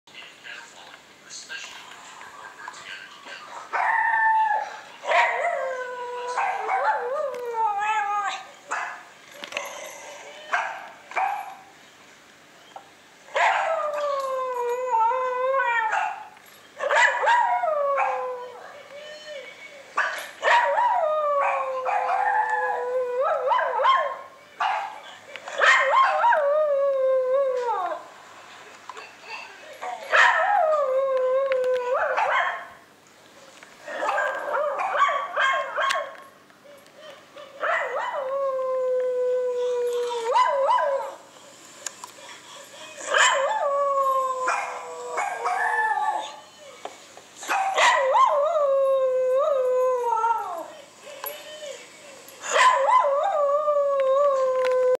Jinx headtilt
Jinx tilting his head at puppies barking on my Ipad